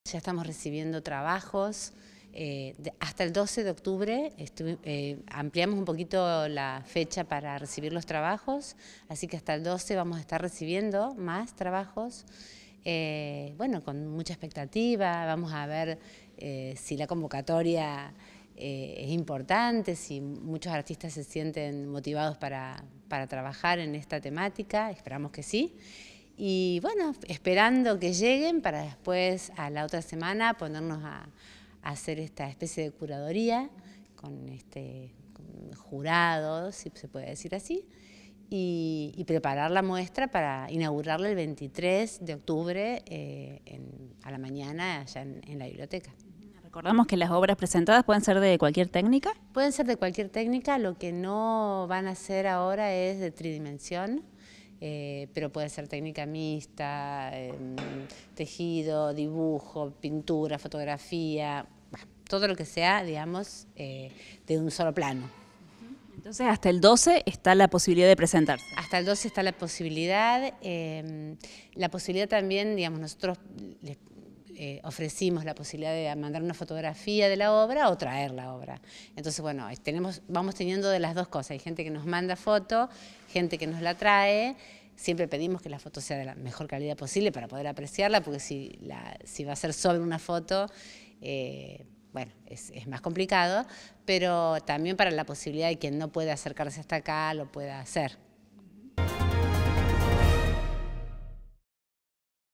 HASTA EL 12 DE OCTUBRE SE RECIBEN PROPUESTAS PARA EL SALÓN DE ARTES VISUALES “40 AÑOS DE DEMOCRACIA”
DANIELA FUENTES – RESPONSABLE DPTO. ARTE Y CULTURA
UniRio TV
Universidad Nacional de Río Cuarto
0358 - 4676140